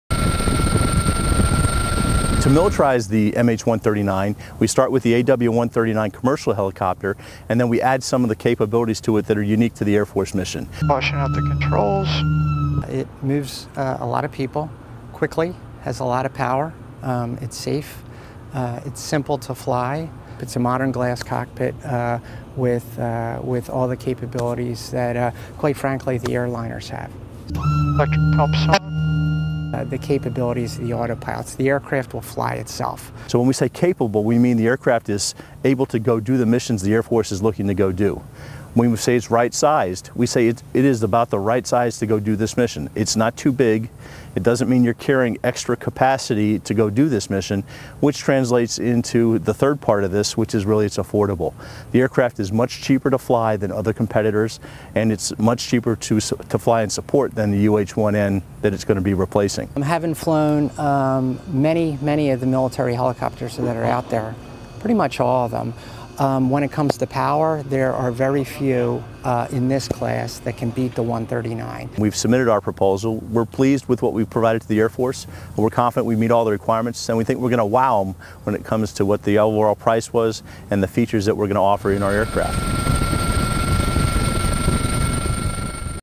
L'elicottero Boeing MH-139 versione militare dell'AW-139
Boeing e la italiana Leonardo Helicopters hanno presentato una proposta alla US Air Force per fornire fino a 84 elicotteri MH-139, versione militare del noto elicottero civile AW-139, a sostituzione degli ormai anziani UH-1N Huey e per proteggere le basi e le infrastrutture che ospitano i missili balistici intercontinentali del paese e per le necessità di trasporto militare interno.